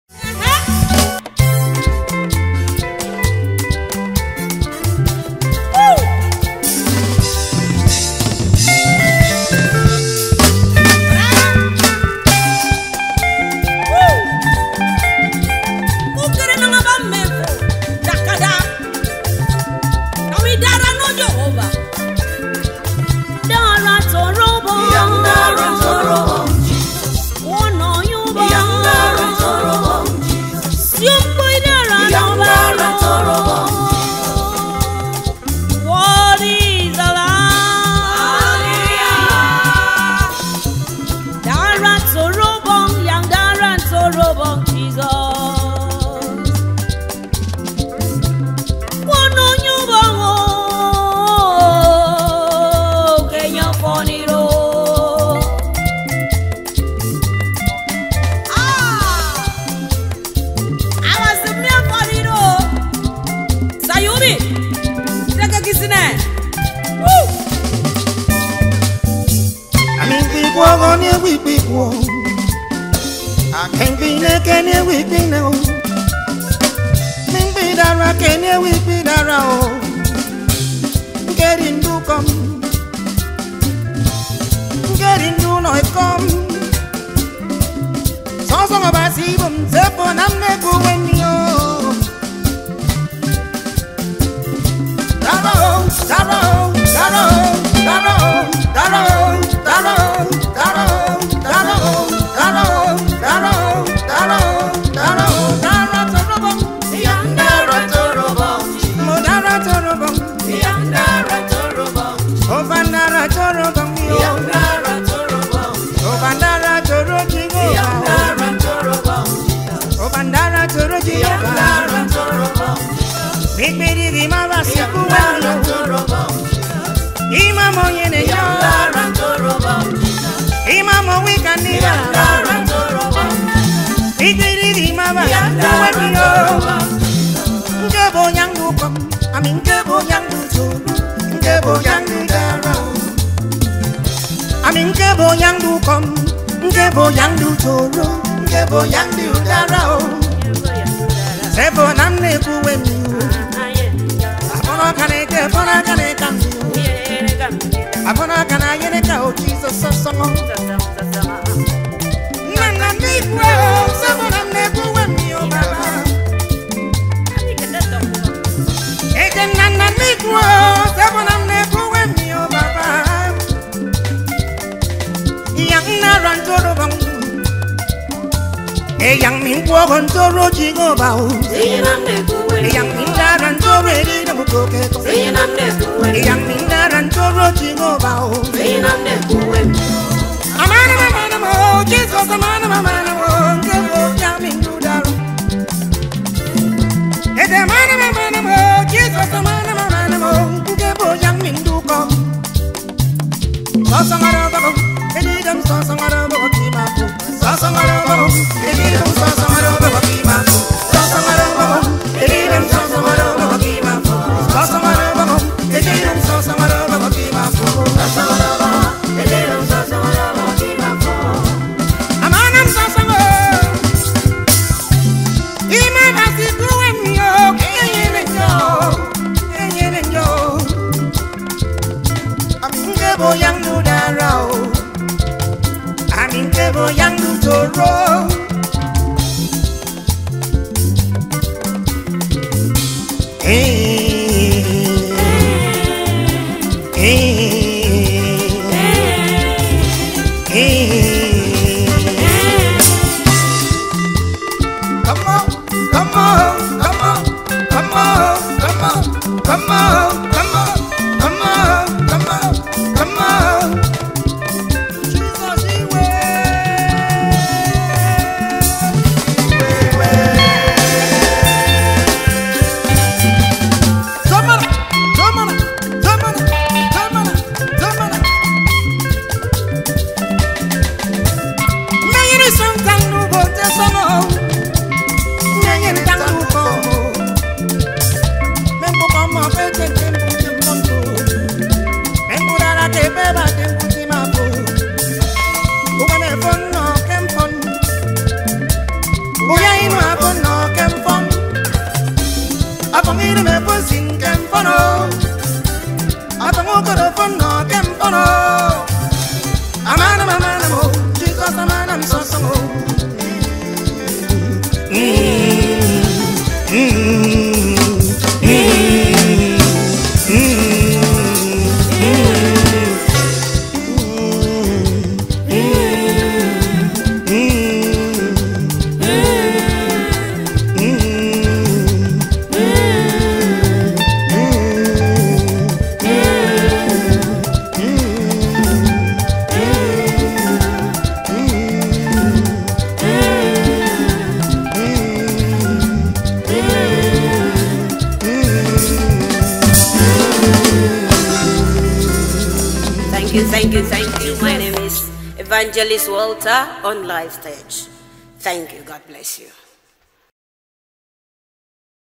Dance to this African Gospel Music - African Tribal Music
The best live band performance in African Gospel Music 2023